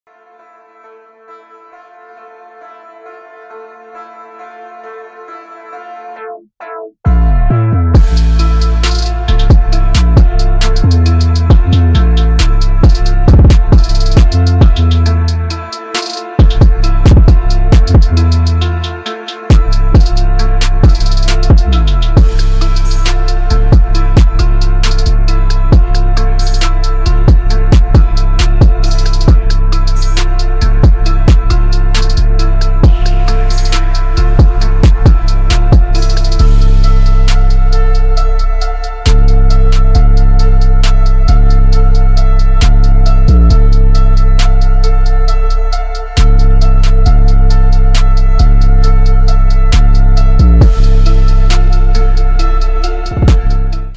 Tesla Model 3 // Konig Wheels // Filmed with a Panasonic GH5
Hey! Here's another video i dug up from almost a year ago. I was working with a buddy of mine who wanted to showcase his new wheel sponsor! Little did i know that this exact car would be featured in SEMA 2019 at the Konig booth. Having to pack up my bags and gear, i was honored to have driven this car from Long Beach, California, to Las Vegas, Nevada, to showcase at SEMA 2019!

This video was filmed entirely handheld with the Panasonic GH5, sadly i was also working on a sprained ankle form a week prior injury. Anyways! Enjoy the video!

Equipment used:
Panasonic GH5

Future Content is currently being worked on and will post up soon. Thank you!